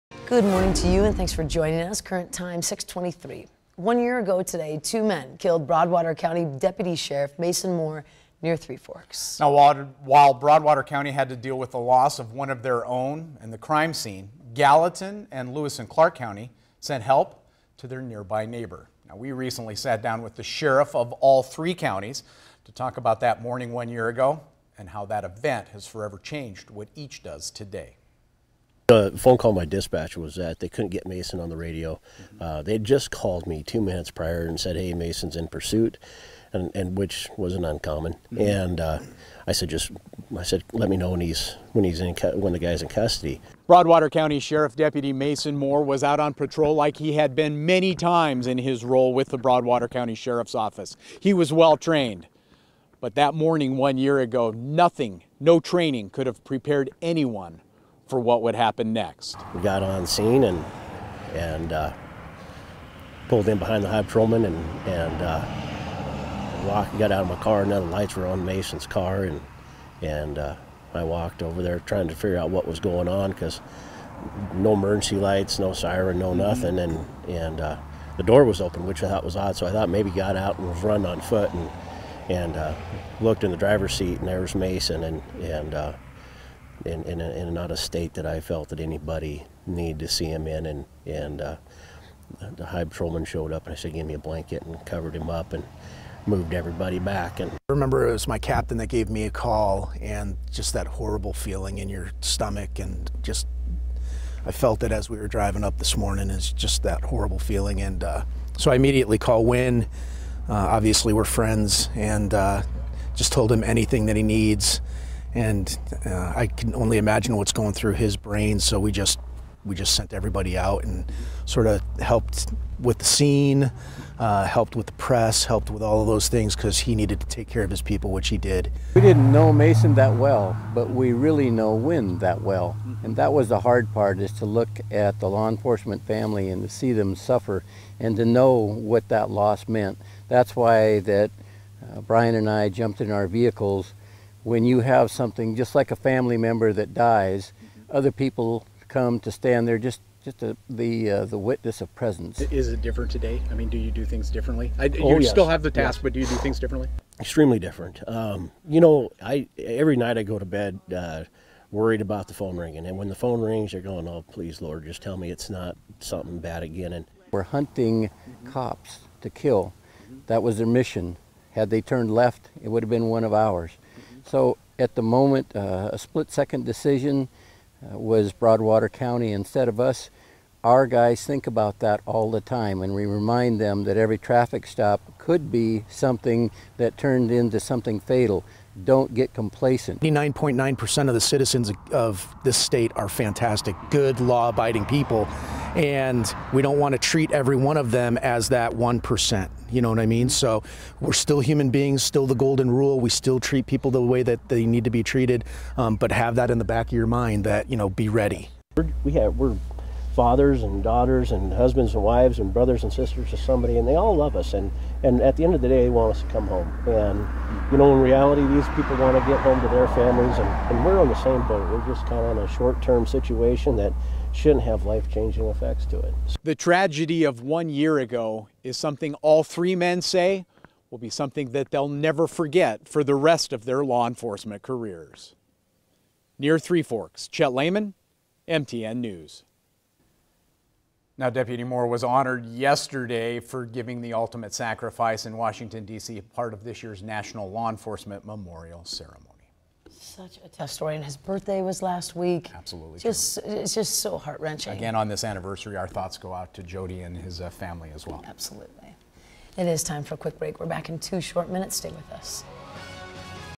Remembering Deputy Mason Moore: One year later
One year ago today two men killed Broadwater County Deputy Sheriff Mason Moore near Three Forks. Broadwater County Sheriff's deputy Mason Moore was out on patrol like he had been many times in his role with the Broadwater county sheriff's office. He was well trained. But that morning, one year ago, nothing, no training could have prepared anyone for what would happen next.